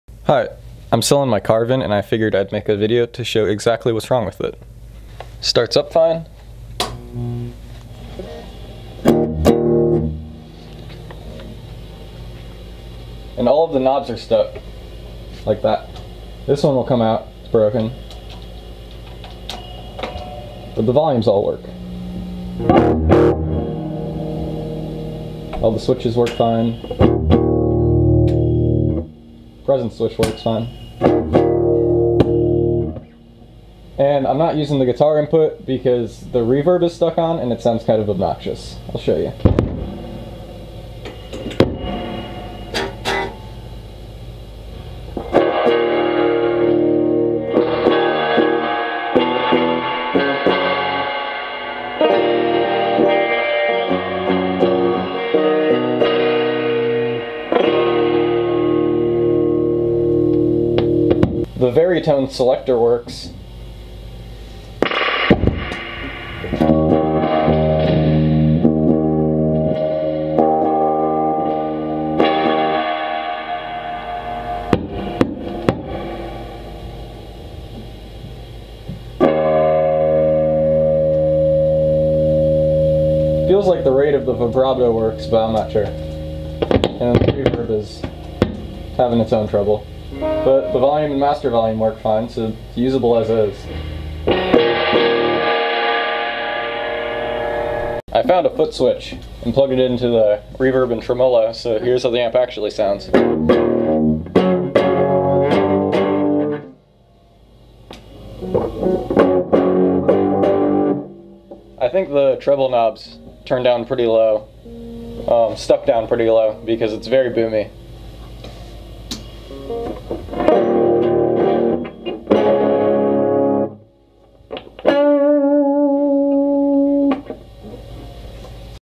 Carvin ST2400 For Sale
Quick video showing the issues with the amp.